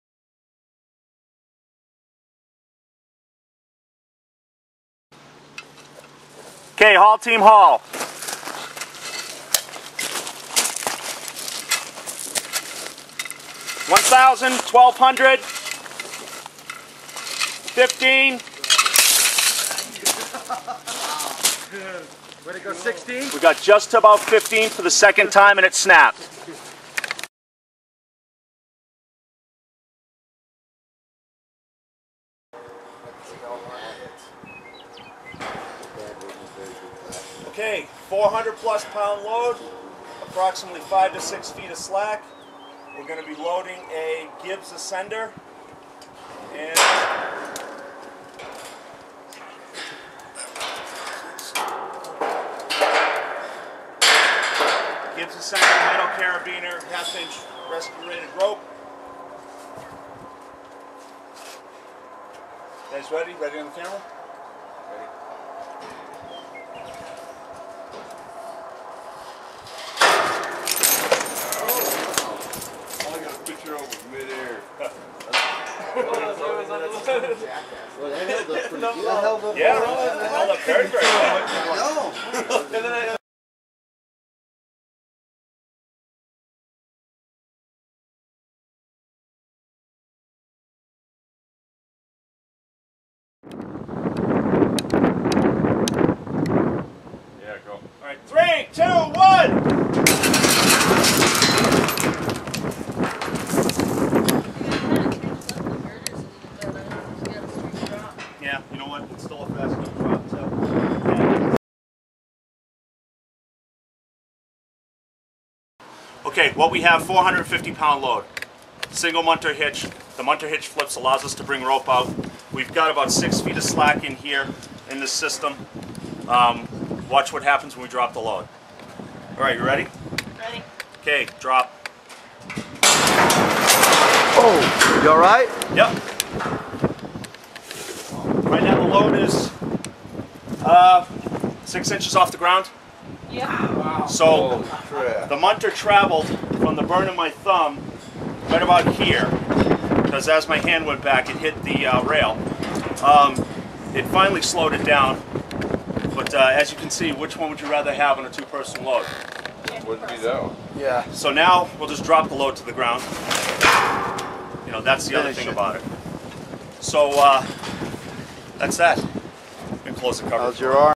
Drop test
this is a drop test with a 400 plus lb load. this test illustrates the need for a belay that can be operated automatically.